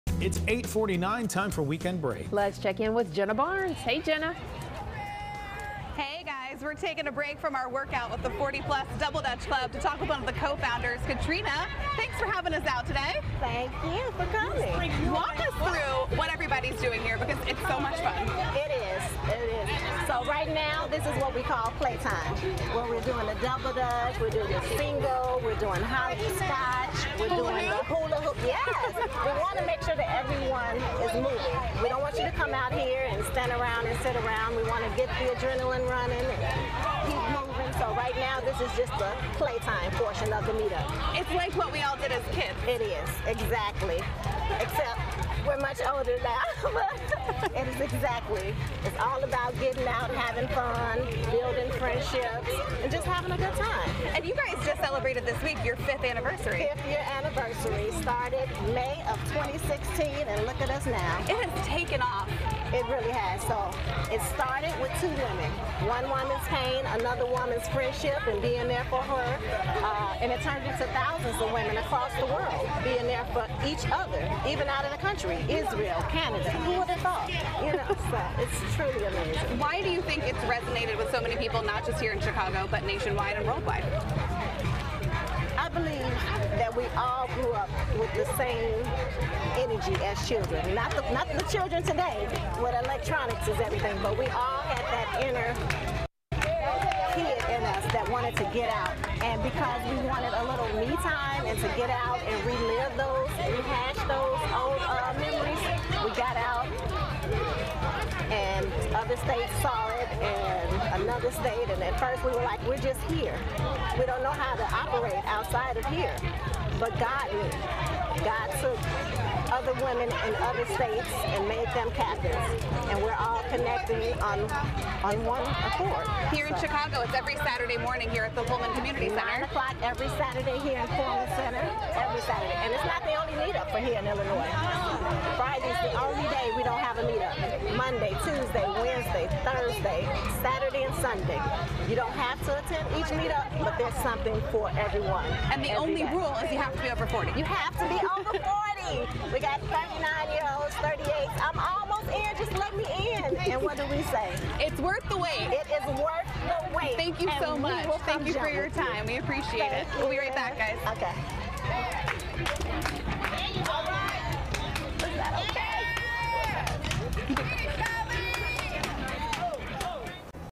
Double Dutch club for women over 40 is proving age is just a number
Every Saturday a group of women meets outside the Pullman Community Center to prove that age is just a number